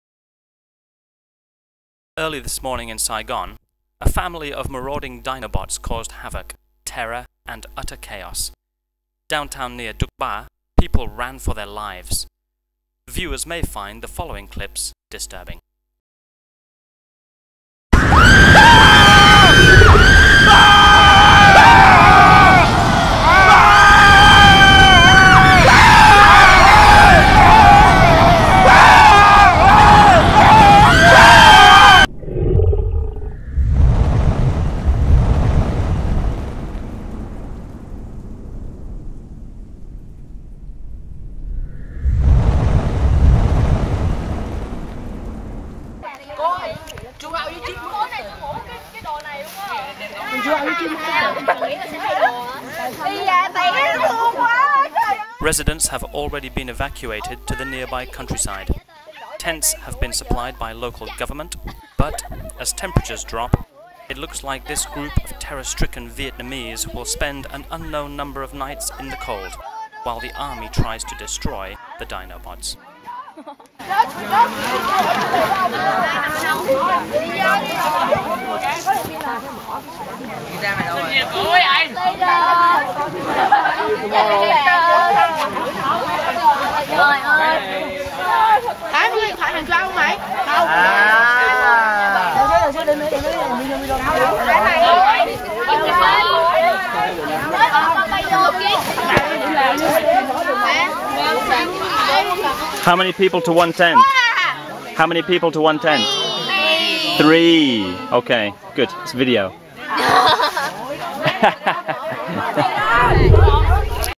Saigon Invasion
This is the beginning of a tragedy that unfolded in Saigon, Vietnam, in early 2013.